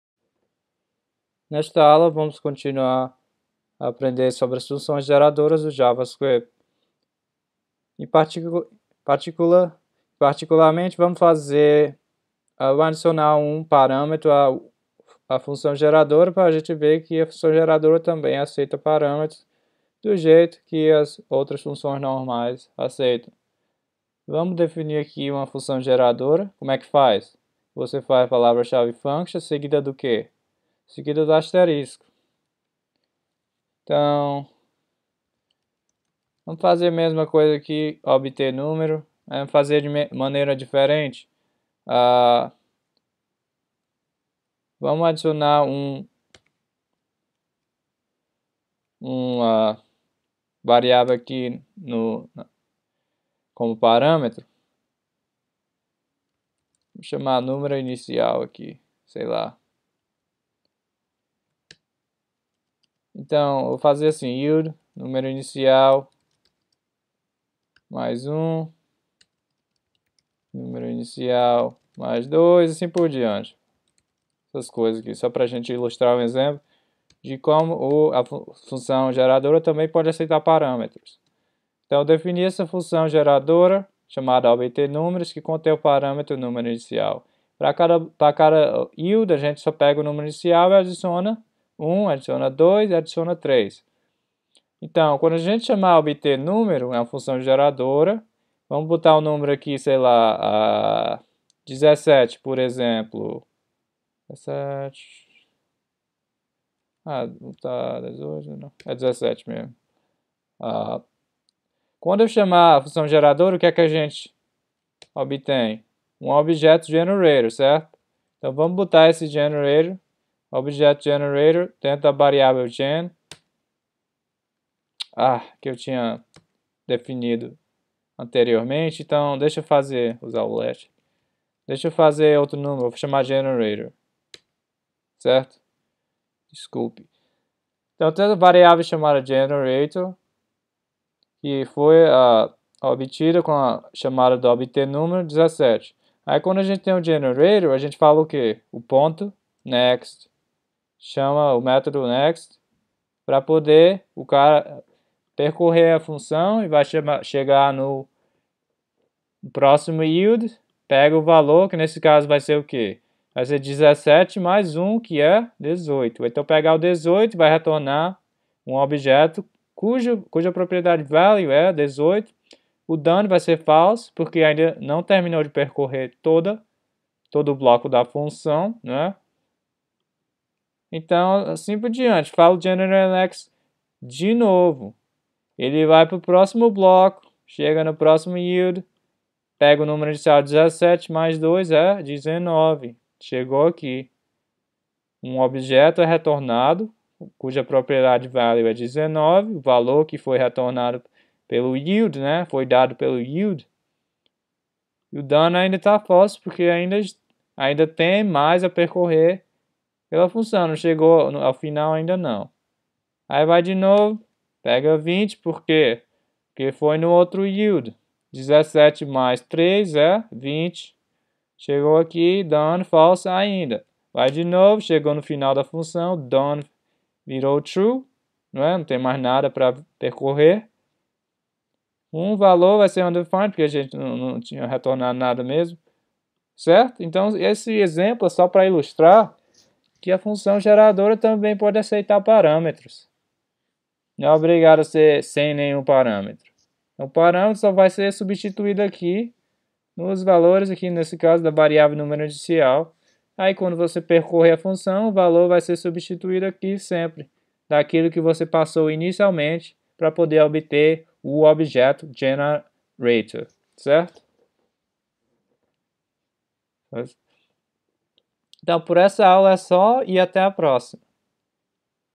000b-02 Funções Geradoras do JavaScript, com Parâmetros
Aprenda sobre as funções geradoras (“generator functions”) do JavaScript. São aquelas que aparecem com a palavra chave function*, seguida de um asterisco.